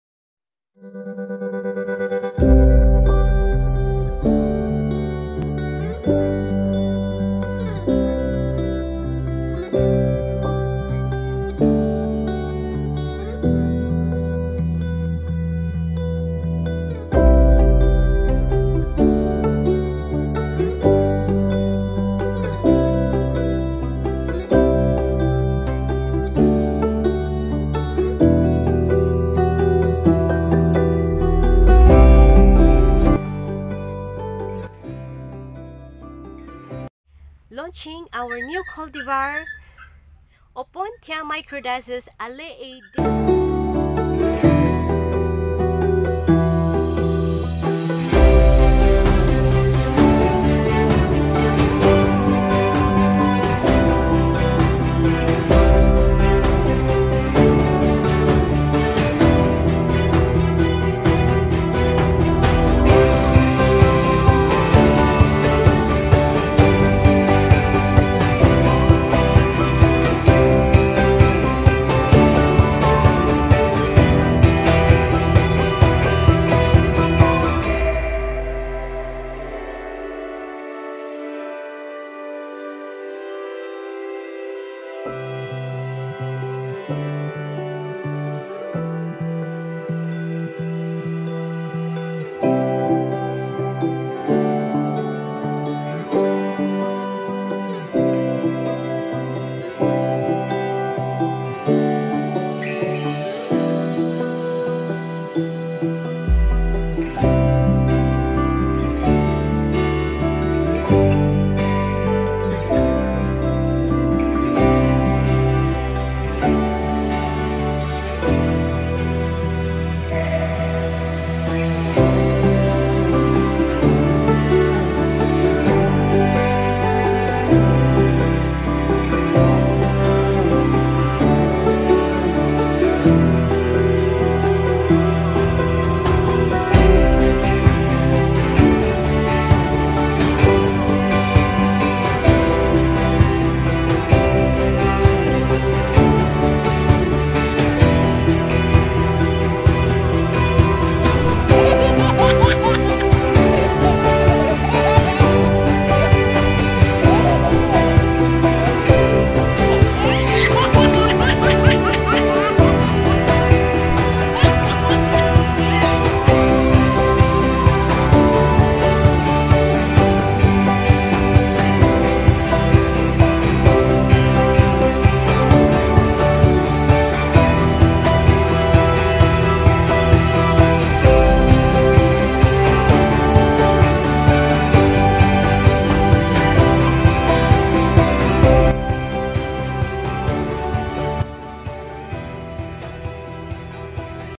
Philippine's New Breakthrough on Cactus World!!!
This is to introduce the very rare Cactus plant discovered at the cold mountainous region of the Cordillera Philippines!

After 5 years of observations and experiments how does this Cactus grow in different climate and how to propagate it, we found it very friendly and not hard to propagate!  After propagating into hundreds of pieces,  We are very to launch it to be able to share to those who wanted to buy their own collection!

Yes! Collecting a new Plant is a very exciting part of life for those Plant enthusiasts!